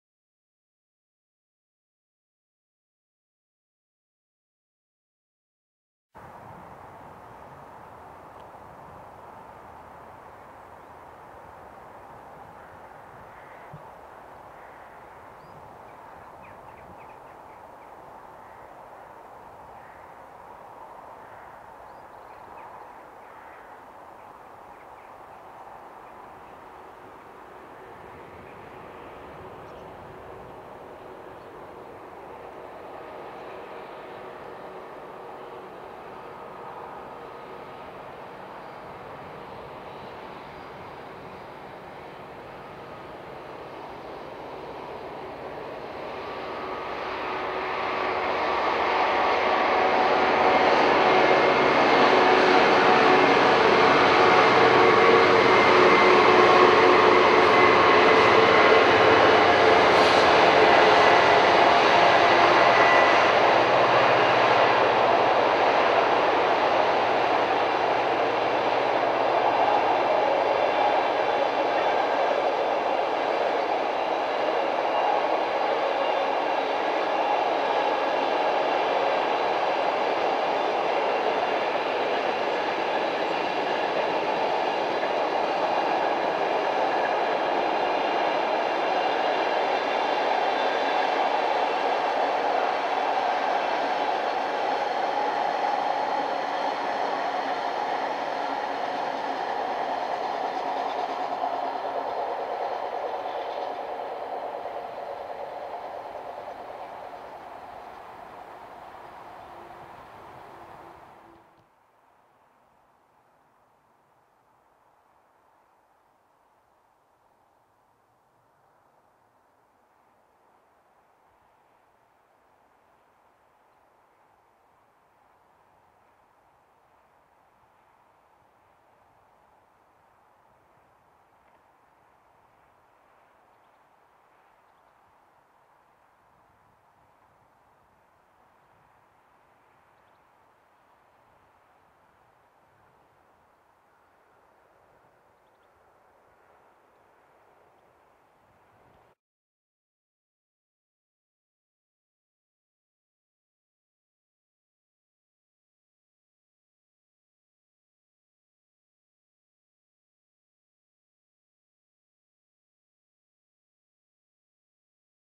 Two 66s on the "Lessco" Liner Grind Up Shap.
Taken about three-quarters of an hour before the famous 60009 came up this famous grade with the commemorative train of the BR's Last Steam Train" event.

Want to see some more like this?  Try the "stanbrinsmead" channel?